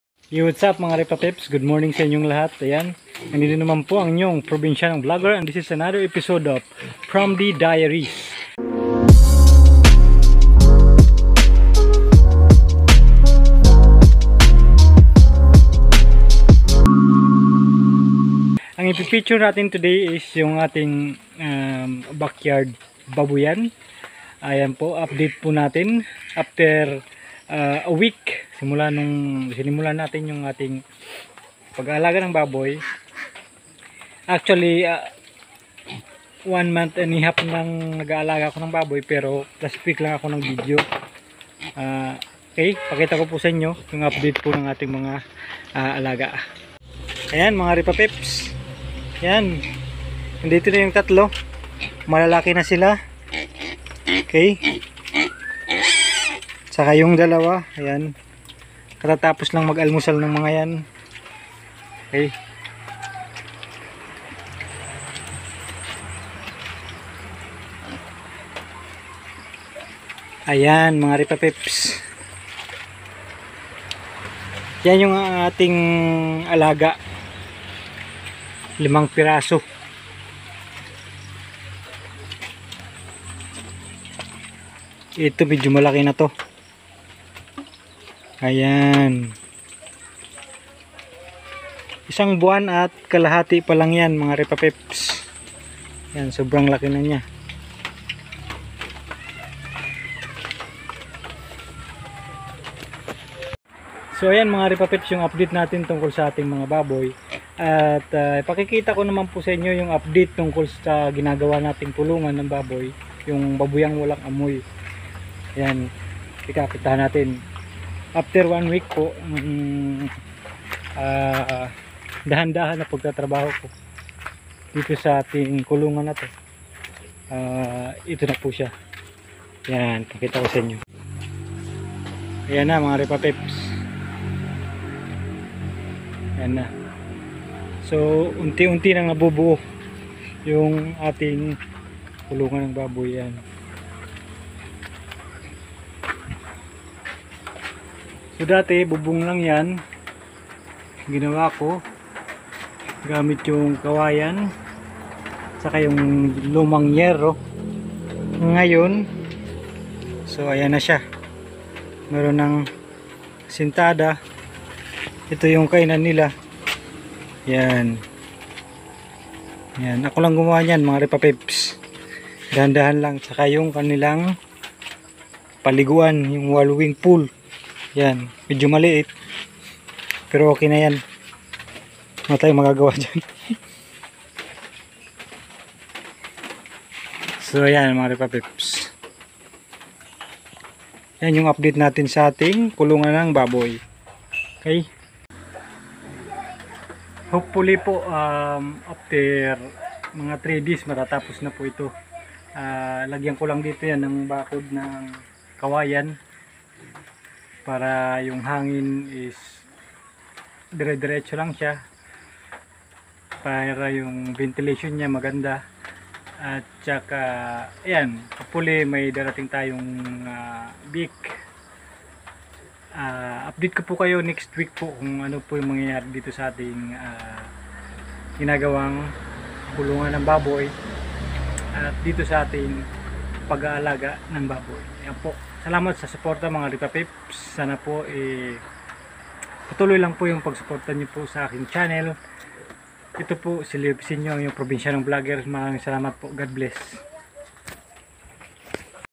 Backyard small piggery.| Baboyang walang amoy | update | promdi diaries
hello everyone welcome to my youtube channel today in this episode is an update on my recently started backyard piggery, and also an update about my little project on building a pig house.